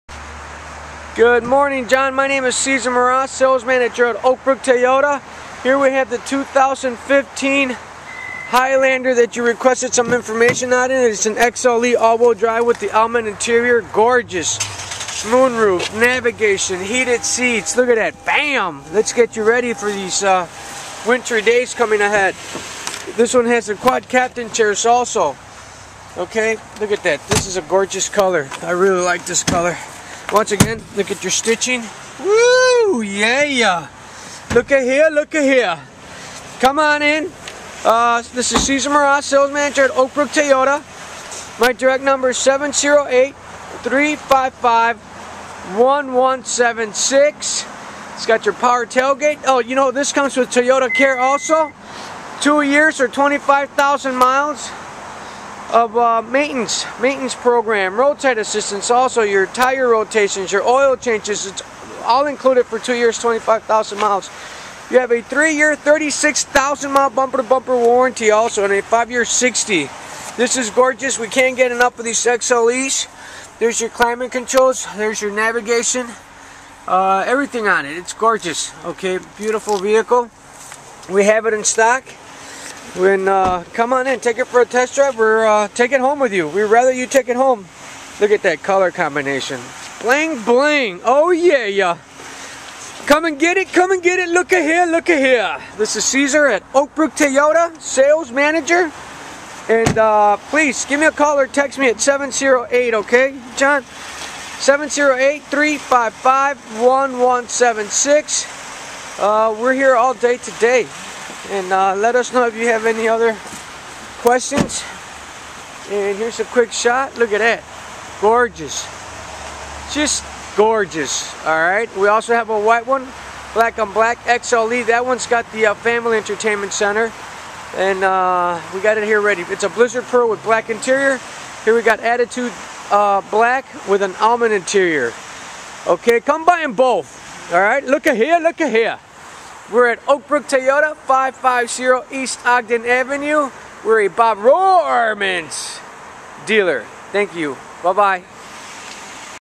2015 Toyota Highlander XLE AWD for John K.
Good morning JK. Please enjoy the video of the 2015 Toyota Highlander XLE AWD that we have in stock and ready for delivery. My name is Cesar Meraz and I'm the Sales Manager at Oakbrook Toyota that will provide you with Excellent Service and a transparent car buying process! Thank You!